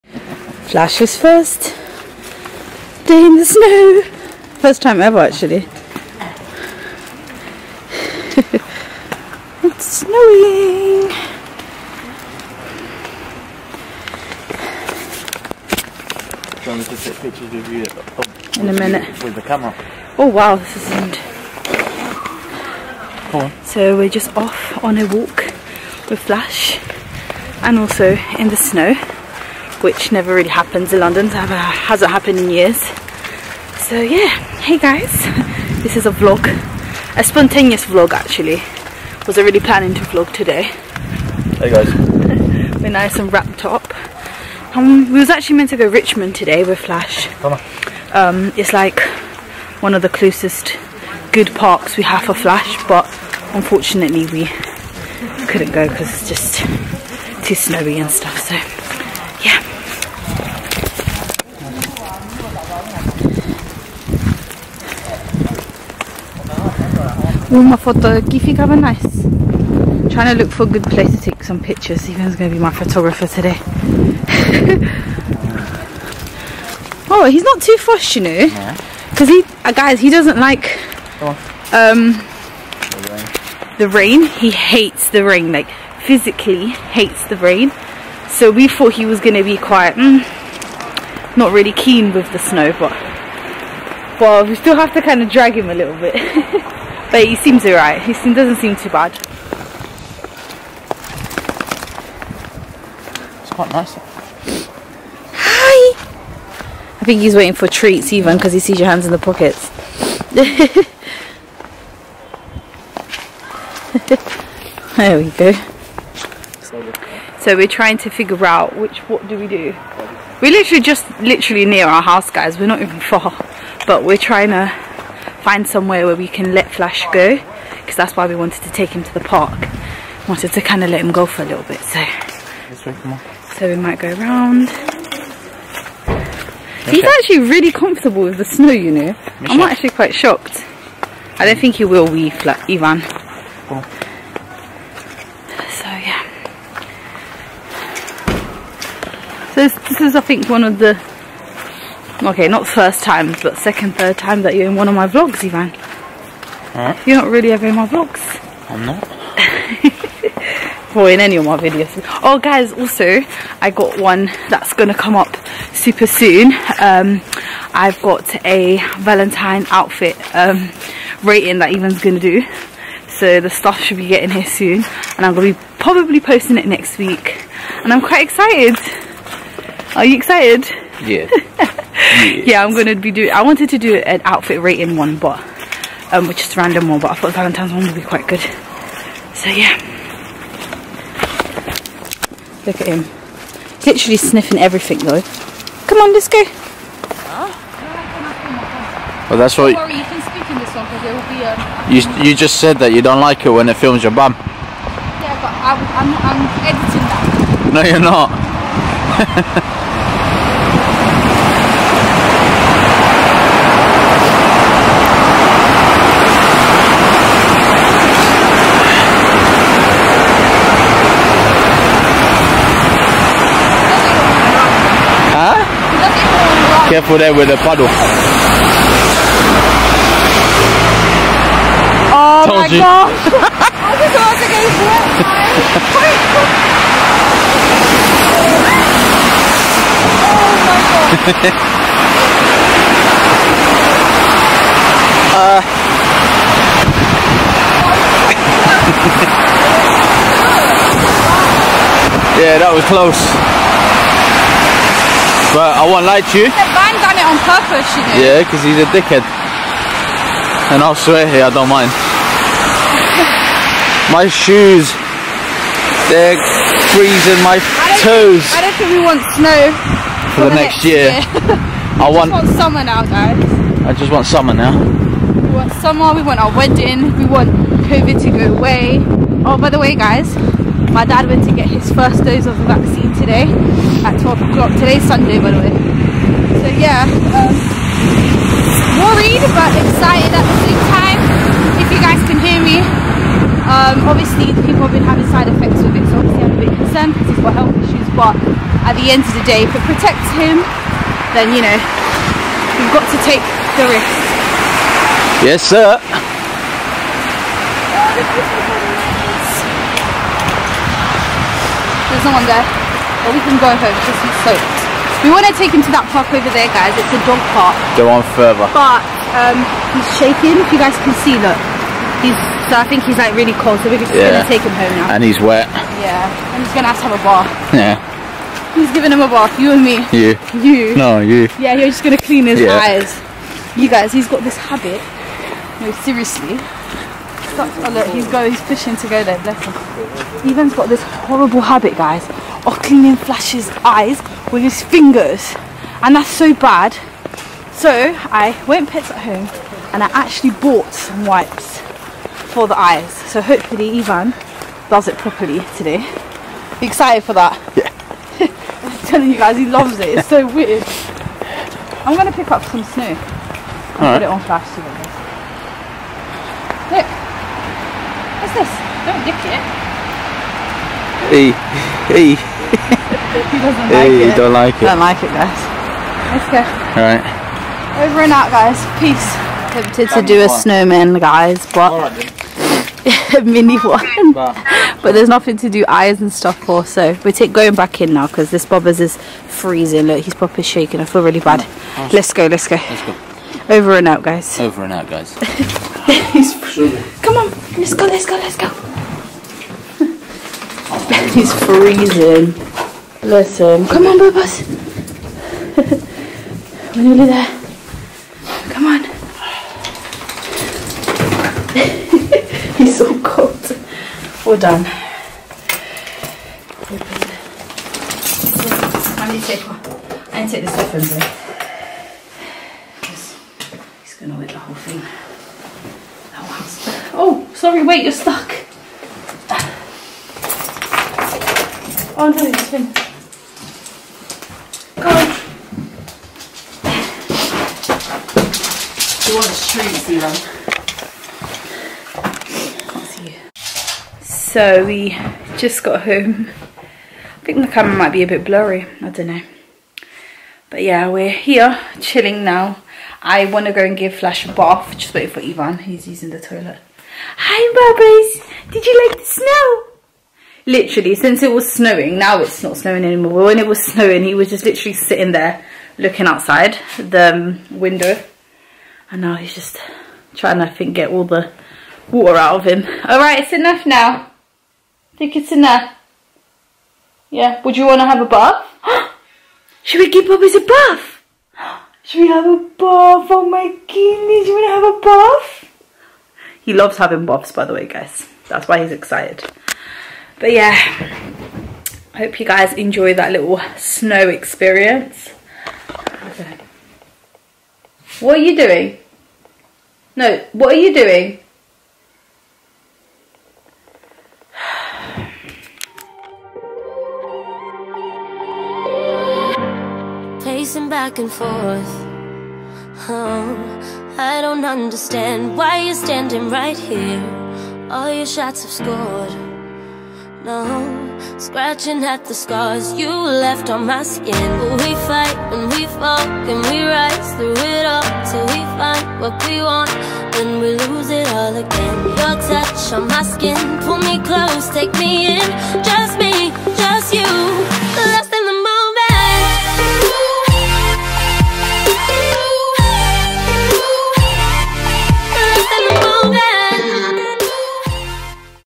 Vlog in the snow in london | Our dogs first time in the snow reaction
This was a spontaneous Vlog in the snow in london

This was one of my winter to do list was to go in the snow and take a picture and i cnat believe it actually happned as it does not always snow in London every year. You also get to see flash's reaction to the snow which was pretty better than I expected.

London looks beautiful when it snows its just a shame it dont stay for long.

This is a small vlog but sweet this is what is fun about doing youtube, its the random video whihc come out the best at times.